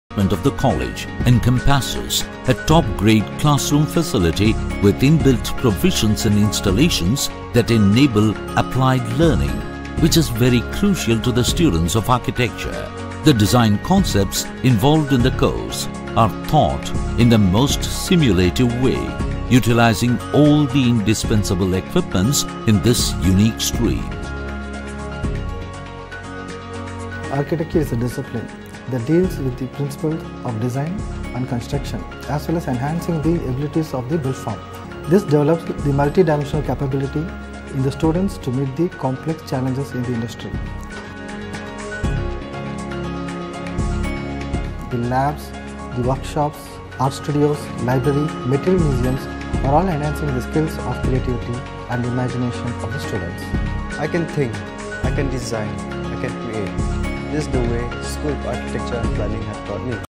School of Architecture and Planning- VMRFSAP
Creation and provision of High quality Professional Education and socially relevant in a wide range of Interdisciplinary areas of Architecture, Planning, Construction Technology, Sustainability, Urban Design to a large number of students from all sections of the society in the country.